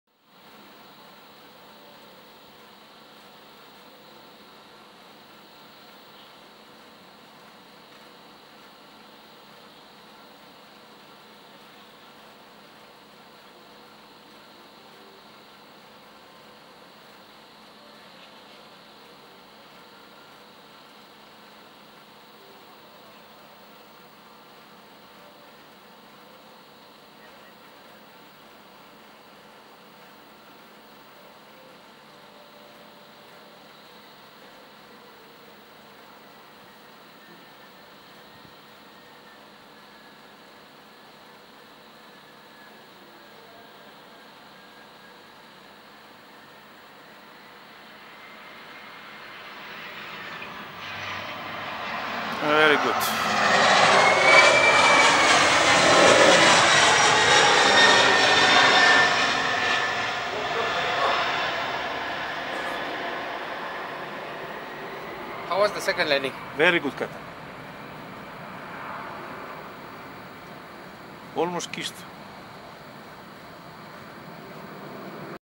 Base training Airbus 319, Paro landing RWY15
First landing RWY 15 in Paro.